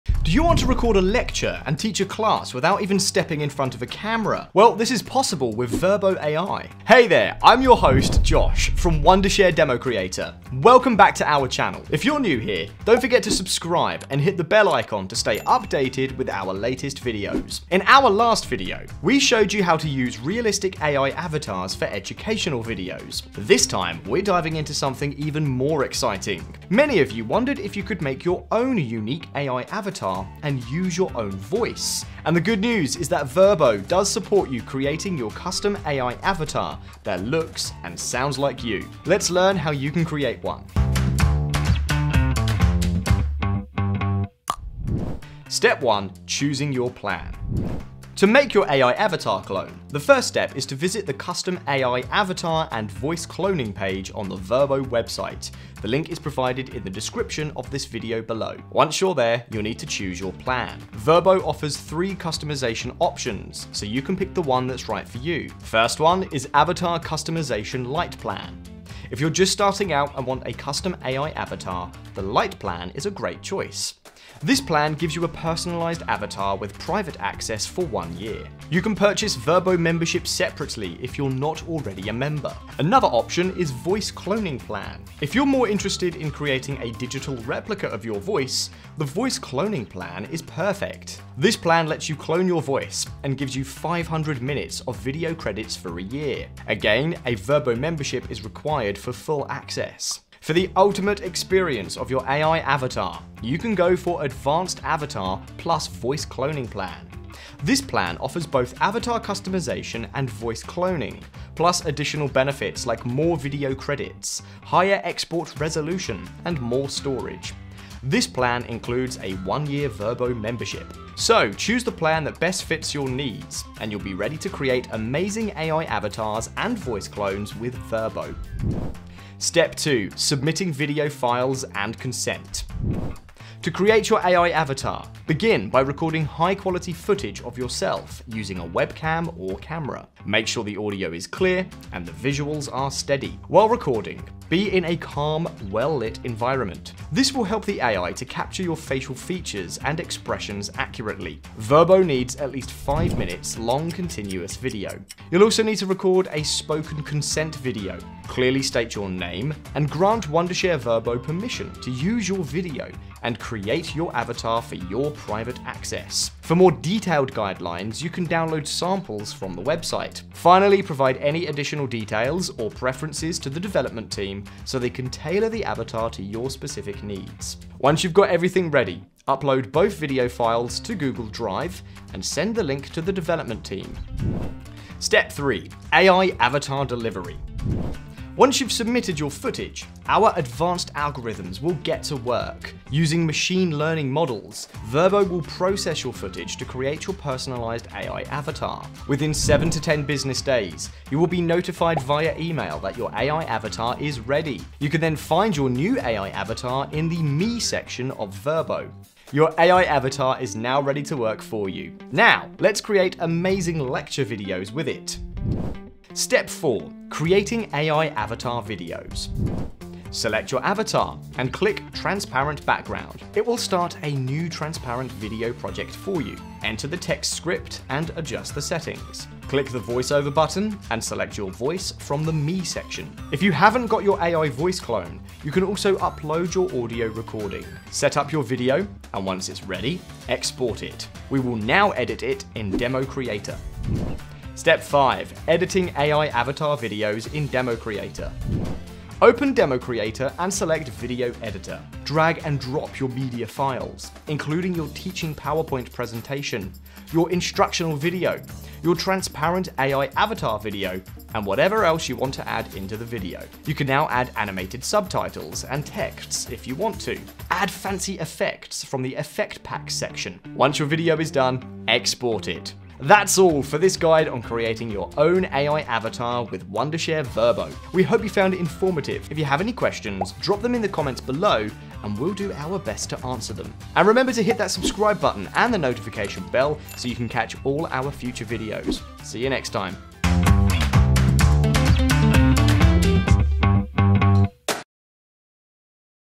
How to Custom Your Own AI Avatars and Create Teaching Videos
In our last video where we showed you how to use realistic AI avatars for educational videos. This time, we're diving into something even more exciting. Many of you wondered if you could make your own unique AI avatar and use your own voice. And the good news is that Virbo does support you creating your custom AI avatar that looks and sounds like you.

⌚ Timestamps
0:00 Introduction
0:46 First step to custom AI avatar
2:10 Submitting video files and consent
3.05 AI Avatar Delivery
3.38 Creating AI Avatar Videos
4:04 Editing AI Avatar Videos in Democreator
4.34 Conclusion